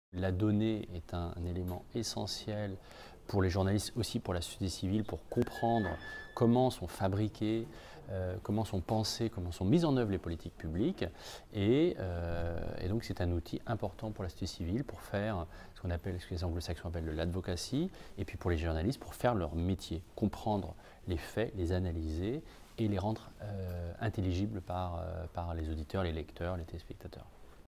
OGP16 - rencontre avec Pierre Jalladeau (Directeur Afrique - CFI)
L'EFJ était avec CFI au Sommet Mondial de l'OGP 2016 (Partenariat pour un Gouvernement Ouvert).

Le journalisme change, nous aussi. Après dix ans d’existence, l’Ecole Française de Journalisme est devenue l’Ecole du nouveau journalisme.